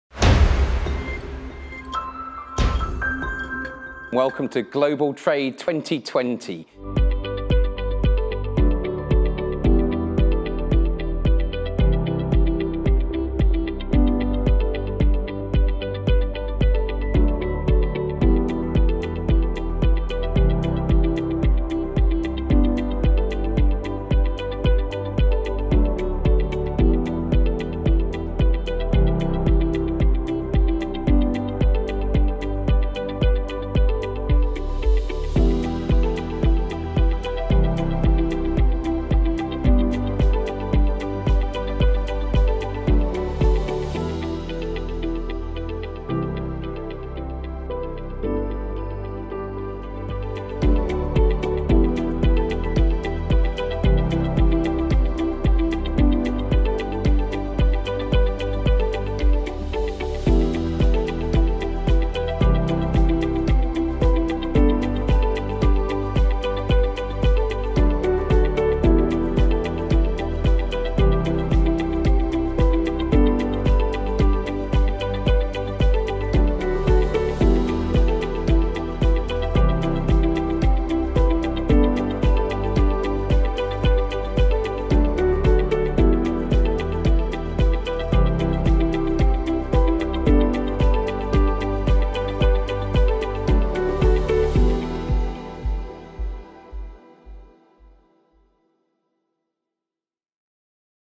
Global Trade Conference 2020 highlights
Watch the highlights of the Greater Birmingham Chambers of Commerce's first digital Global Trade Conference.